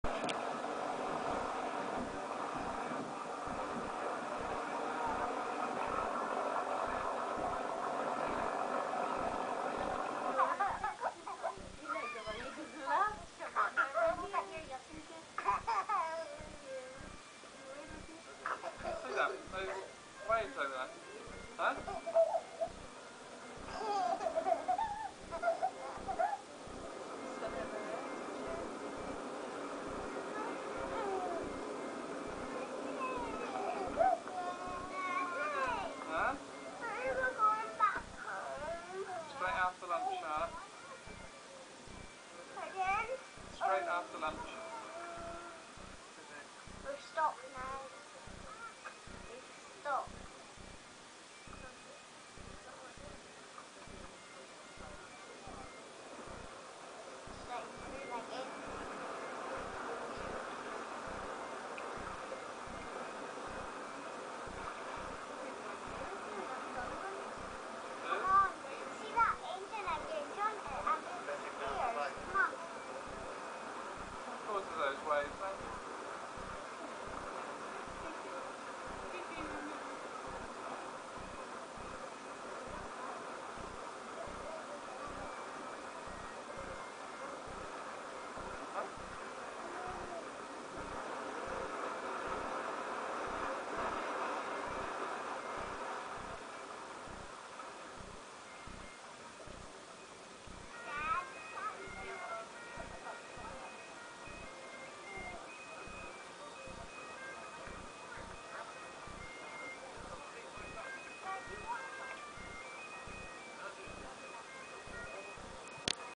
Loch Ness Monster film 1996 sighting Urquart Castle Inverness
On a family outing on tour boat moored beneath Urquart Castle on Loch Ness while videoing the panorama across the Loch something large and dark is seen moving left to right beneath the surface and breaking the water into white waves.
Reported at the time by both Sky News and the Inverness Courier
Filmed by me in 1996